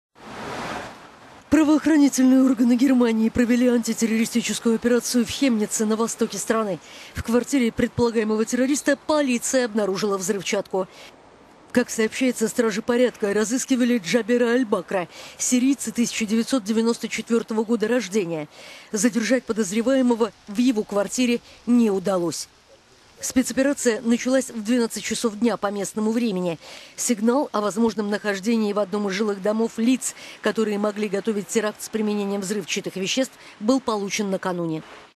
Полиция Германии провела антитеррористическую операцию
Правоохранительные органы Германии провели антитеррористическую операцию в Хемнице на востоке страны. В квартире предполагаемого террориста полиция обнаружила взрывчатку.

Спецоперация началась в 12 часов дня по местному времени. Сигнал о возможном нахождении в одном из жилых домов лиц, которые могли готовить теракт с применение взрывчатых веществ, был получен накануне.

Сообщается, что полиция разыскивала Джабера Альбакра, сирийца 1994 года рождения. Задержать подозреваемого в его квартире н…